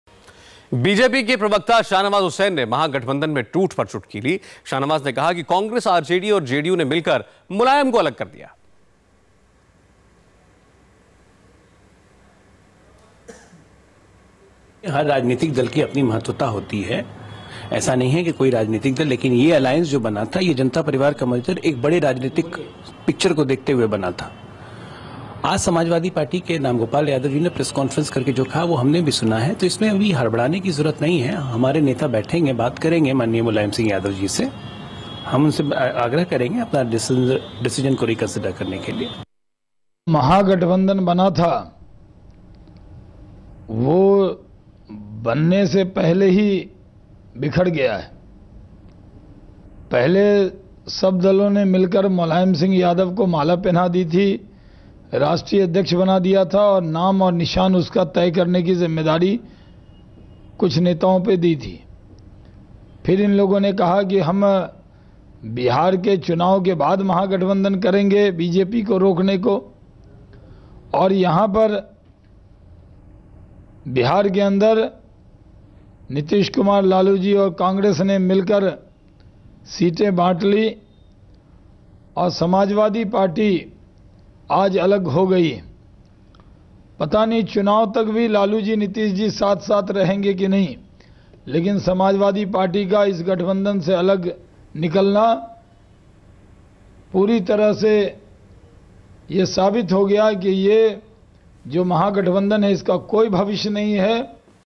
SP's Exit Proves Grand Alliance Has No Future, says Shahnawaz Hussain - India TV
Samajwadi Party's exit proves grand alliance has no future, says BJP leader Shahnawaz Hussain.